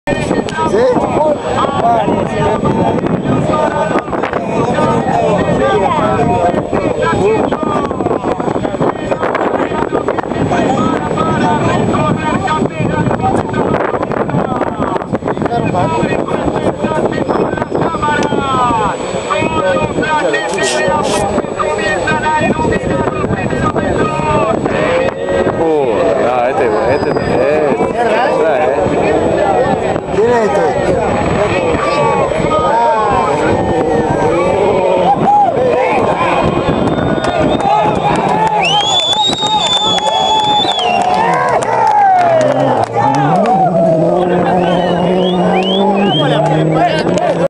WR2008 EN CAPILLA DEL MONTE CIRCUITO PERILAGO
IMAGENES TOMADAS POR JUAN CALDERATO EN EL CIRCUITO PERILAGO DEL DIQUE EL CAJON EN 2008